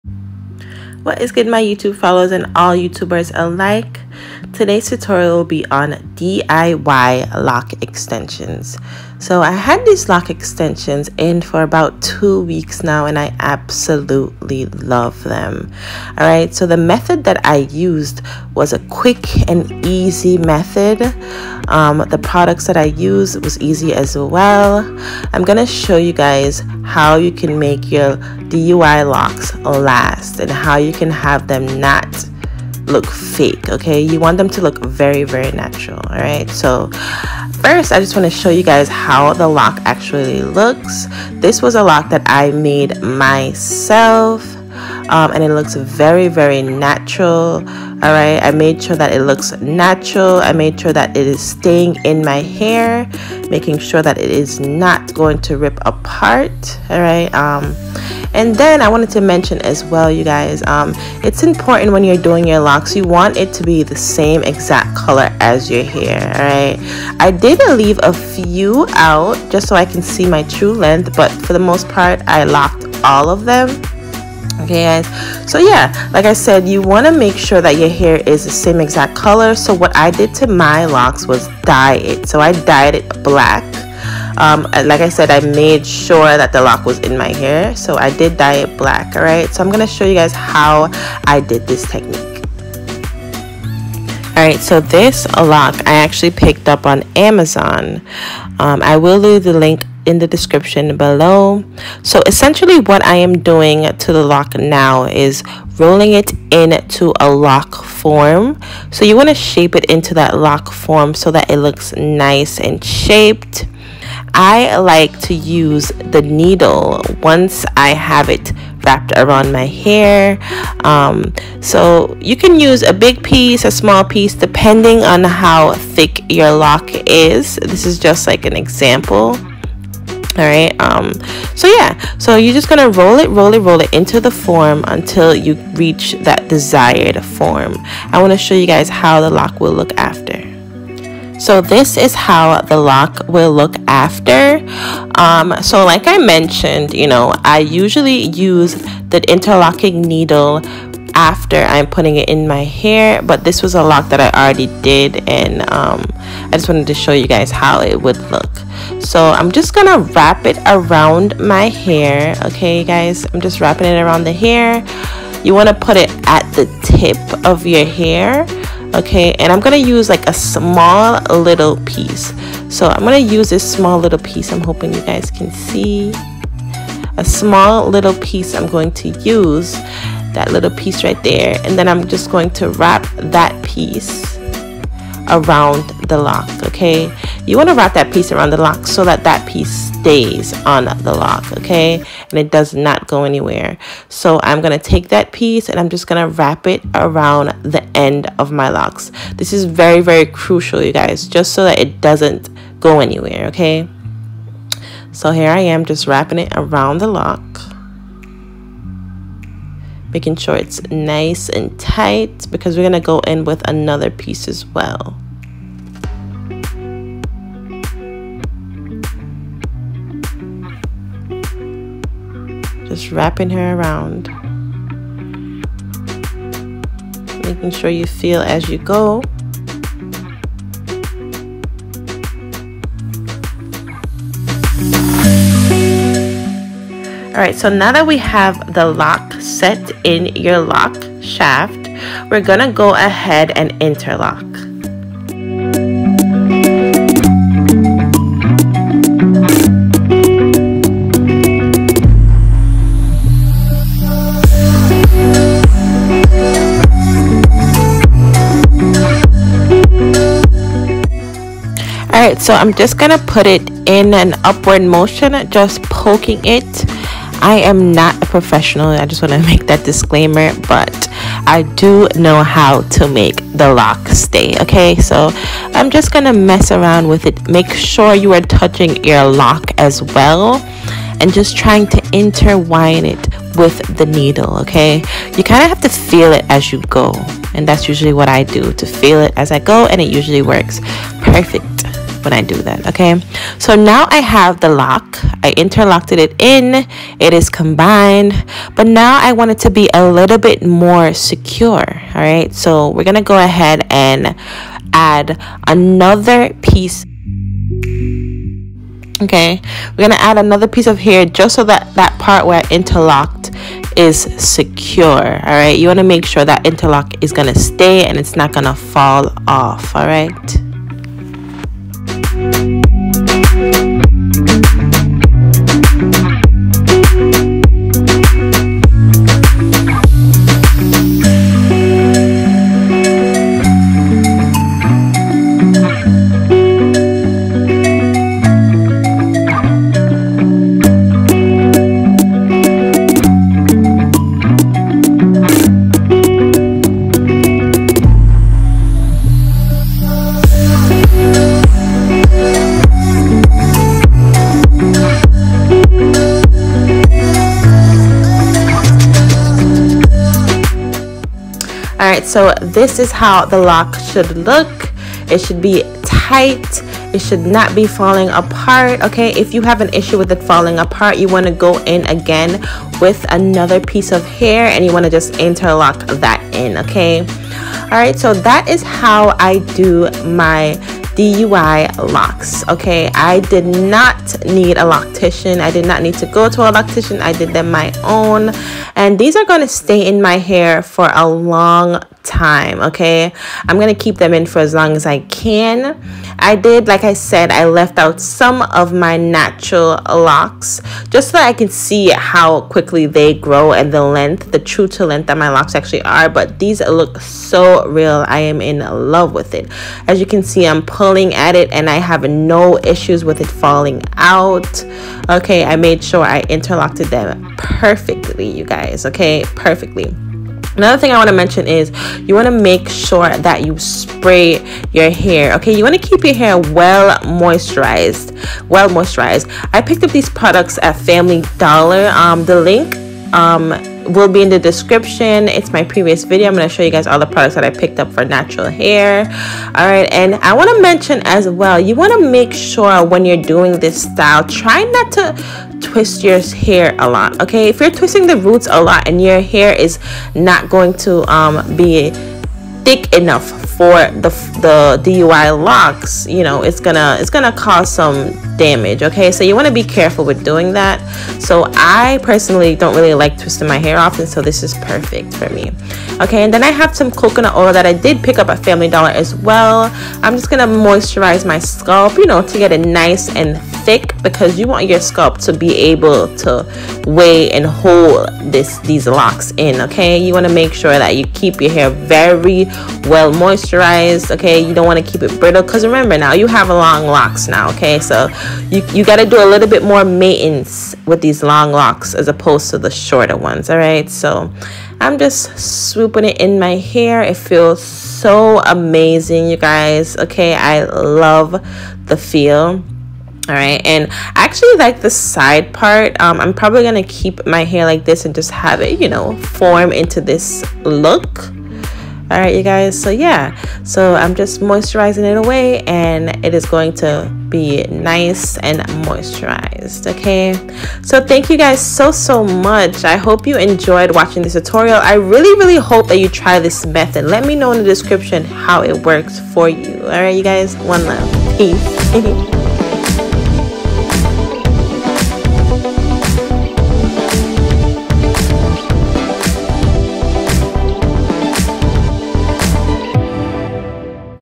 DIY LOC EXTENSIONS| ADDING LENGTH TO MY LOCS| THE EASIEST METHOD!
Hey loves are you interested in adding length to your loc babies and creating locs of your own? This method is the easiest and most natural method for adding locs to your hair! No stiff method. 🤎

Link to my Hair and all favorite LOC products ✨(interlock needle included)

Inspirational videos✨